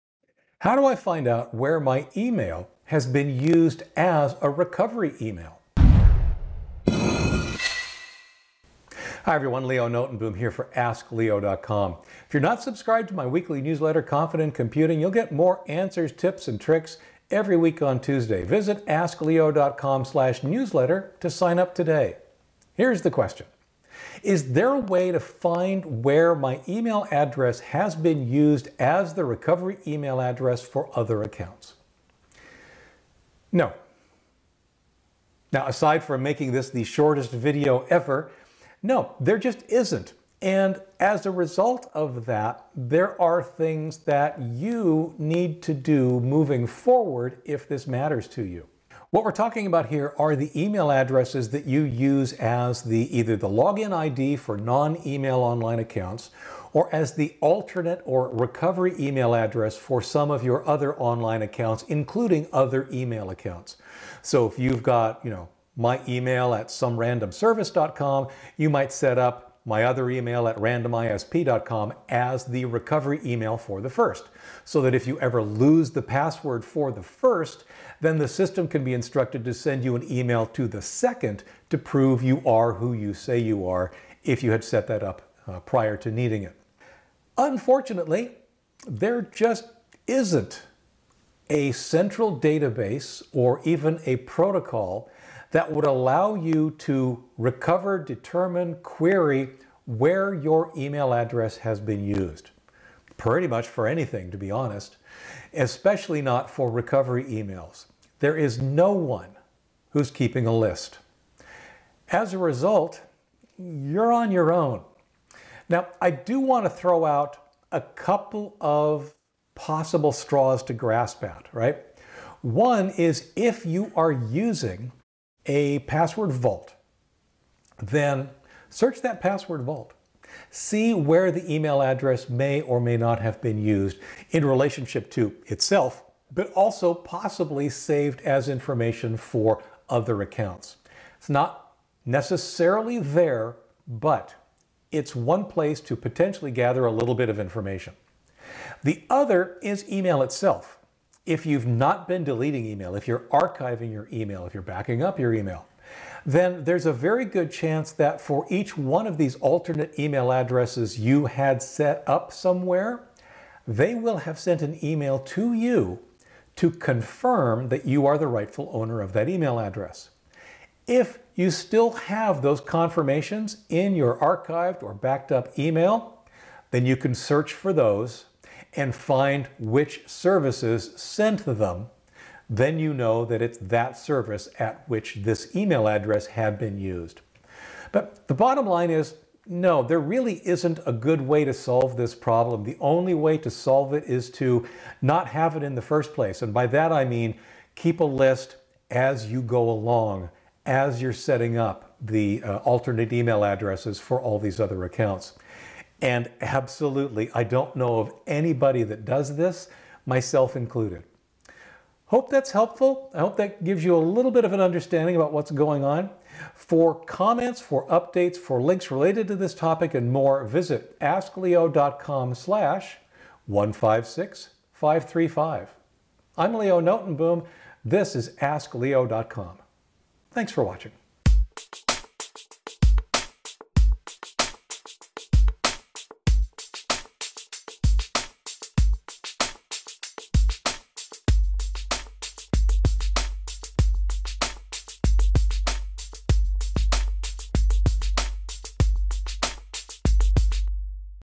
How Do I Find Where My Email Has Been Used as a Recovery Email?
❌ Recovery email used where?
A password vault and your own email archives might be able to determine some, but the only way to get a complete list is to keep such a list yourself as you go.

Chapters
0:00 Find My Recovery Email
0:50 What is a Recovery email
2:00 A password vault may help